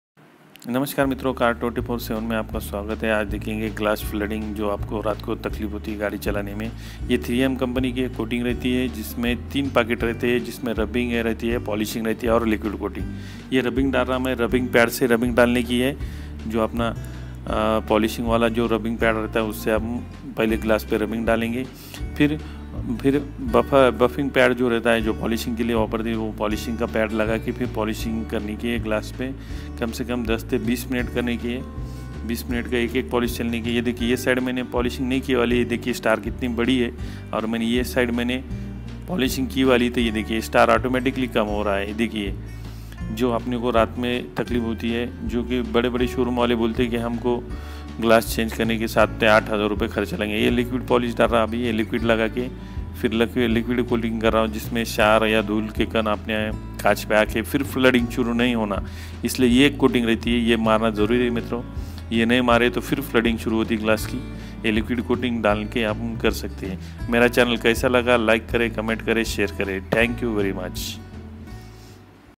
glass repairing only in 1000 rupees
car windshield glass flooding in only 1000 ₹ Language Hindi